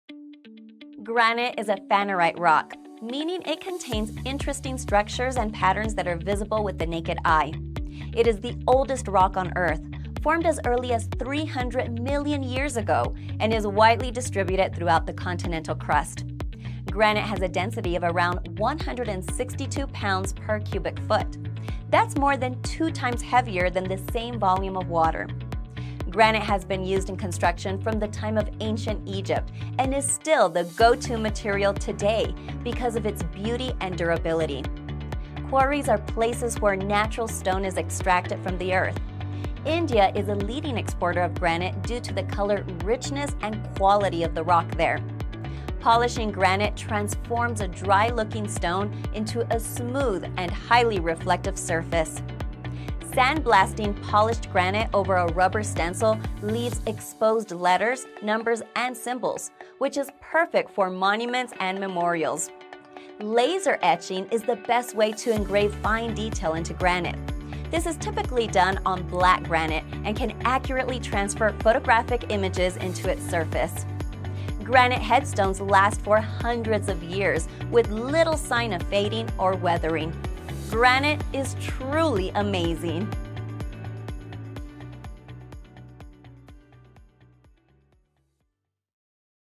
Granite is Amazing!
Why is granite an amazing rock to use in monuments and memorials? This intro to granite should help answer that.